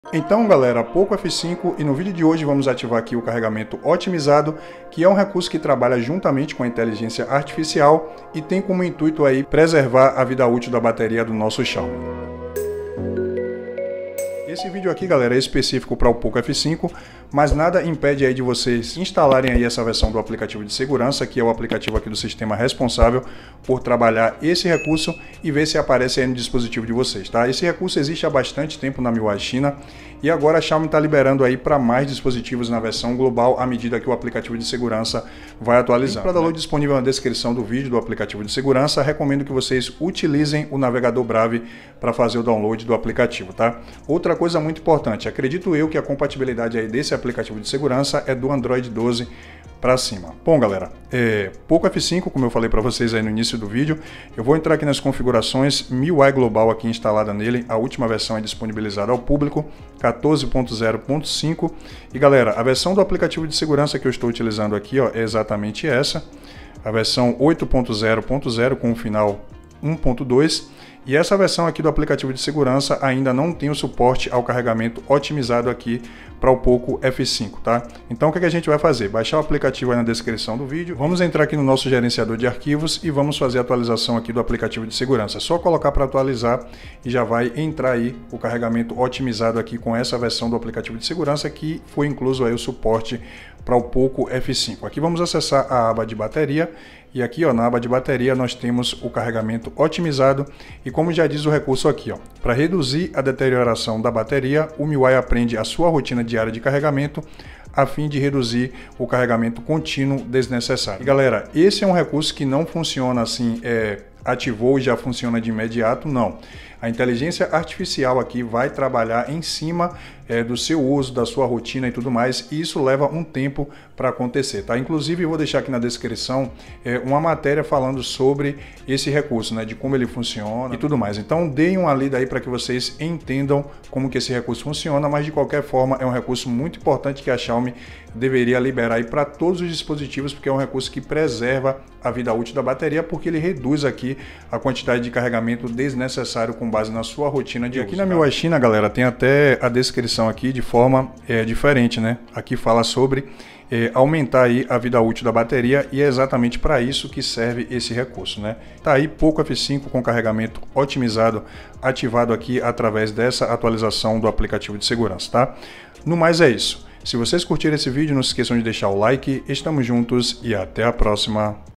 Xiaomi Liberou - Ative Agora! Carregamento Otimizado Poco F5 (Pode Funcionar em Outros dispositivos)
Como funciona o carregamento otimizado